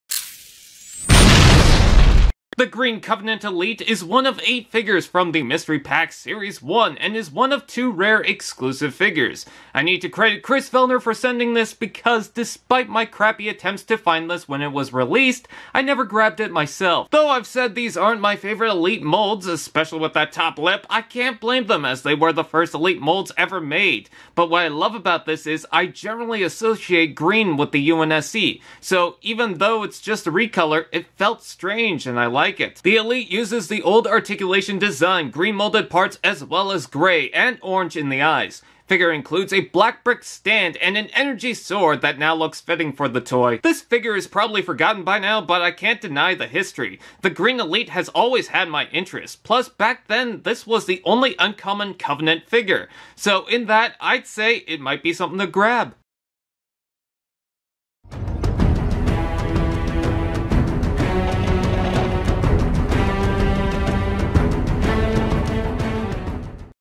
📷 Mega Bloks HALO: Mystery Pack Series #1: Covenant Elite (Green) || DOOM Construx Review
JOIN US:
Add to your collection, and create new adventures with the Halo Series 1 Hero Packs. These packs include a variety of common, rare and ultra rare characters from the Halo Universe! The Green Covenant Elite is Rare in the set.